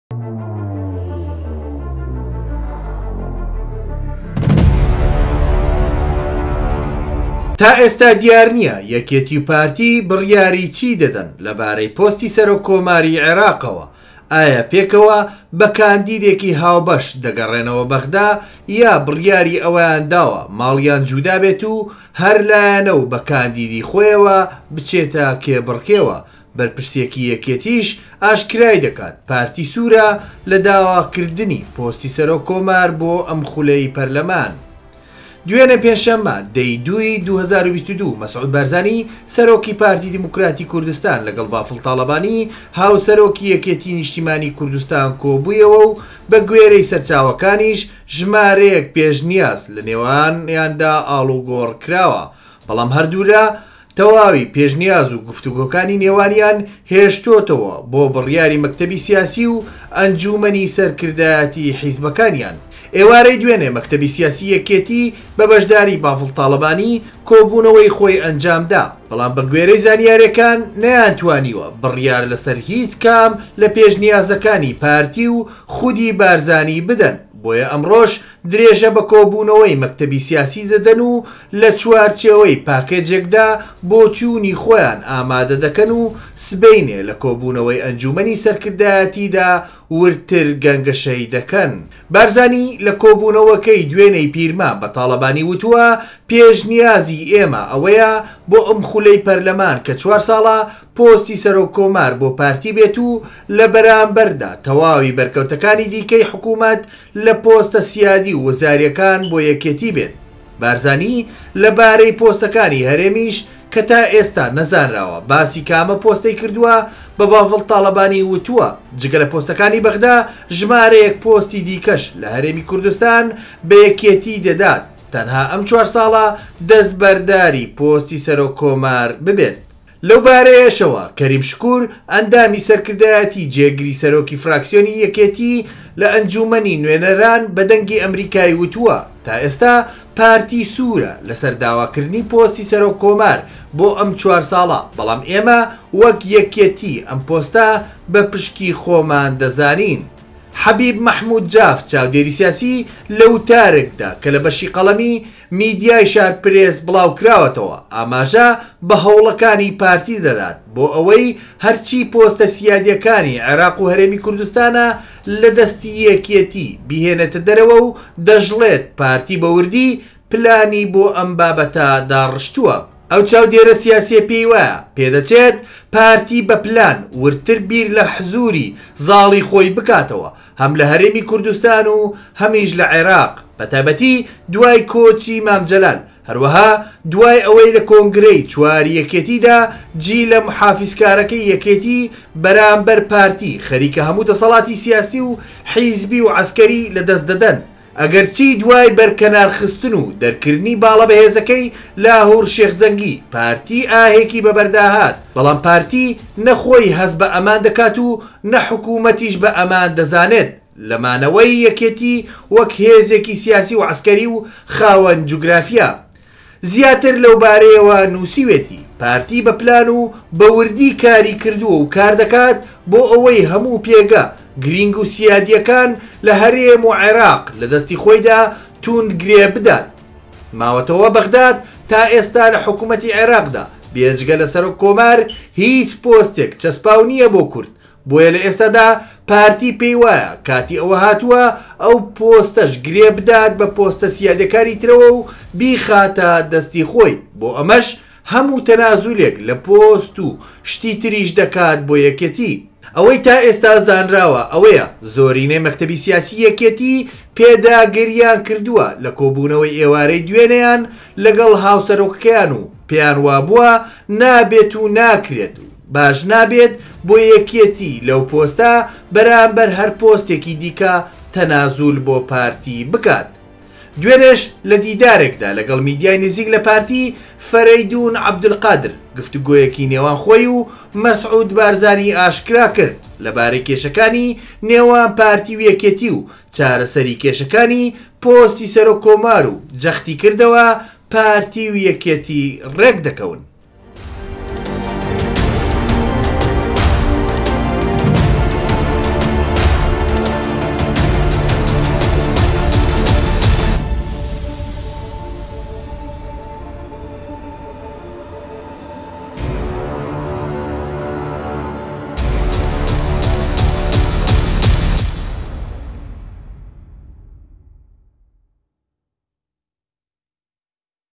هات 🔴 بزانە پارتی پلانی چی هەیە بەرامبەر یەکیەتی 😱
تائێستا دیارنییه‌ یه‌كێتى‌و پارتى بڕیارى چى ده‌ده‌ن له‌باره‌ى پۆستى سه‌رۆك كۆمارى عێراقه‌وه‌ ئایا پێكه‌وه‌ به‌كاندیدێكى هاوبه‌ش ده‌گه‌ڕێنه‌وه‌ به‌غدا، یان بڕیارى ئه‌وه‌یانداوه‌ ماڵیان جودابێت‌و هه‌رلایه‌نه‌و به‌كاندیدى خۆیه‌وه‌ بچێته‌ كێبڕكێوه‌، به‌رپرسێكى یه‌كێتیش ئاشكرایده‌كات، پارتى سوره‌ له‌داواكردنى پۆستى سه‌رۆك كۆمار بۆ ئه‌م خوله‌ى په‌رله‌مان.

دوێنێ پێنجشەمە 10ى 2ى 2022 مەسعود بارزانی سەرۆکی پارتی دیموکراتی کوردستان لەگەڵ بافڵ تاڵەبانی هاوسەرۆکی یەکێتی نیشتیمانی كوردستاندا کۆبۆوه‌و به‌گوێره‌ى سه‌رچاوه‌كانیش، ژماره‌یه‌ك پێشنیاز له‌نێوانیاندا ئاڵوگۆڕ كراوه‌، به‌ڵام هه‌ردولا ته‌واوى پێشنیازو گفتوگۆكانى نێوانیان هێشتۆته‌وه‌ بۆ بڕیارى مه‌كته‌بى‌و سیاسى‌و ئه‌نجومه‌نى سه‌ركردایه‌تى حزبه‌كانیان.

ئێواره‌ى دوێنێ مه‌كته‌بى سیاسى یه‌كێتى به‌به‌شدارى بافڵ تاڵه‌بانى كۆبونه‌وه‌ى خۆى ئه‌نجامدا، به‌ڵام به‌گوێره‌ى زانیاریه‌كانى شارپرێس، نه‌یانتوانیوه‌ بڕیار له‌هیچكام له‌پێشنیازه‌كانى پارتى‌و خودى بارزانى بده‌ن، بۆیه‌ ئه‌مڕۆش درێژه‌ به‌كۆبونه‌وه‌ى مه‌كته‌بى سیاسى ده‌ده‌ن‌و له‌چوارچێوه‌ى پاكێجێكدا بۆچونى خۆیان ئاماده‌ ده‌كه‌ن‌و سبه‌ینێ له‌كۆبونه‌وه‌ى ئه‌نجومه‌نى سه‌ركردایه‌تیدا وردتر گه‌نگه‌شه‌ى ده‌كه‌ن.

 بارزانى له‌كۆبونه‌وه‌كه‌ى دوێنێى پیرمام به‌تاڵه‌بانى وتوه‌، پێشنیازى ئێمه‌ ئه‌وه‌یه‌ بۆ ئه‌م خوله‌ى په‌رله‌مان كه‌ چوارساڵه‌، پۆستى سه‌رۆك كۆمار بۆ پارتى بێت‌و له‌به‌رامبه‌ردا ته‌واوى به‌ركه‌وته‌كانى دیكه‌ى حكومه‌ت له‌پۆسته‌ سیادى‌و وزاریه‌كان بۆ یه‌كێتى بێت.

بارزانى له‌باره‌ى پۆسته‌كانى هه‌رێمیش كه‌ تائێستا نه‌زانراوه‌ باسى كامه‌ پۆسته‌ى كردوه‌، به‌بافڵ تاڵه‌بانى وتوه‌، جگه‌ له‌پۆسته‌كانى به‌غدا، ژماره‌یه‌ك پۆستى دیكه‌ش له‌هه‌رێمى كوردستان به‌یه‌كێتى ده‌دات ته‌نها ئه‌م چوارساڵه‌ ده‌ستبه‌ردارى پۆستى سه‌رۆك كۆمار بێت.

له‌وباره‌یه‌شه‌وه‌ کەریم شکور ئه‌ندامى سه‌ركردایه‌تى‌و جێگری سەرۆکی فراکسیۆنی یەکێتی لەئه‌نجومه‌نى نوێنه‌ران به‌ده‌نگى ئه‌مریكاى وتوه‌، "تائێستا پارتی سوره‌ له‌سه‌ر داواكردنى پۆستی سەرۆک کۆمار بۆ ئه‌م چوارساڵه‌، به‌ڵام ئێمە وەکو یەکێتی ئەو پۆستە بەپشکی خۆمان دەزانین".

حەبیب مەحمود جاف چاودێرى سیاسیى له‌وتارێكدا كه‌ له‌به‌شى قه‌ڵه‌مى شارپرێسدا بڵاوكراوه‌ته‌وه‌ ئاماژه‌ به‌هه‌وڵه‌كانى پارتى ده‌دات بۆ ئه‌وه‌ى هه‌رچى پۆسته‌ سیادیه‌كانى عێراق‌و هه‌رێمى كوردستانه‌ له‌ده‌ستى یه‌كێتى بێنێته‌ ده‌ره‌وه‌و ده‌شڵێت:"پارتى به‌وردى پلانى بۆ ئه‌وه‌ داڕشتوه‌".

ئه‌و چاودێره‌ سیاسییه‌ پێیوایه‌، "پێدەچێت پارتی بەپلان وردتر بیر لەحزوری زاڵی خۆی بکاتەوە هەم لەهەرێمی کوردستان‌و هەم لەعێراق، بەتایبەتی دوای کۆچی مام جه‌لال، هەروەها دوای ئەوەی لەکۆنگرەی چوارى یه‌كێتیدا، جیلە موحافزکارەکەی یەکێتی بەرامبەر پارتی خەریکە هەموو دەسەڵاتی سیاسی‌و حزبی‌و عەسکەری لەدەستدەدەن، ئەگەرچی دوای بەرکەنارخستن‌و دەرکردنی باڵە بەهێزەکەی لاهور شێخ جەنگی، پارتی ئاهێکی بەبەرداهات، بەڵام پارتی نە خۆی هەست بەئەمان دەکات‌و نە حکومەتیش بەئەمان دەزانێت لەمانەوەی یەکێتی وەکو هێزێکی سیاسی‌و عەسکەری‌و خاوەن جوگرافیا".

زیاتر له‌وباره‌یه‌وه‌ نوسیویه‌تى:"پارتى بەپلان‌و بەوردی کاریکردوەو کاردەکات بۆ ئەوەی هەموو پێگە گرنگ‌و سیادیەکان لەهەرێم‌و عێراق لەدەستی خۆیدا توند گرێبدات، ماوەتەوە بەغدا تائێستا لەحکومەتی عێراقدا بێجگە لەسەرۆک کۆمار هیچ پۆستێک چەسپاو نییە بۆ کورد، بۆیە لەئێستادا پارتی پێیوایە کاتی ئەوە هاتوە ئەو پۆستەش گرێبدات بەپۆستە سیادیەکانی ترەوەو بیخاتە دەستی خۆی، بۆ ئەمەش هەموو تەنازولێک لەپۆست‌و شتی تریش دەکات بۆ یەکێتی".

ئه‌وه‌ى تائێستا زانراوه‌ ئه‌وه‌یه‌، زۆرینه‌ى مه‌كته‌بى سیاسى یه‌كێتى پێداگریان كردوه‌ له‌كۆبونه‌وه‌ى ئێواره‌ى دوێنێیان له‌گه‌ڵ هاوسه‌رۆكه‌كه‌یان‌و پێیانوابوه‌، نابێت‌و ناكرێت‌و باش نابێت بۆ یه‌كێتى له‌و پۆسته‌ به‌رامبه‌ر هه‌ر پۆستێكى دیكه‌ ته‌نازول بۆ پارتى بكرێت.

دوێنێش له‌دیدارێكدا له‌گه‌ڵ میدیاى نزیك له‌پارتى، فه‌ره‌یدون عه‌بدولقادر گفتوگۆیه‌كى نێوان خۆیی‌و مه‌سعود بارزانى ئاشكراكرد له‌باره‌ى كێشه‌كانى نێوان پارتى‌و یه‌كێتى‌و چاره‌سه‌رى كێشه‌كانى پۆستى سه‌رۆك كۆمارو جه‌ختیكرده‌وه‌، "پارتى‌و یه‌كێتى ڕێكده‌كه‌ون".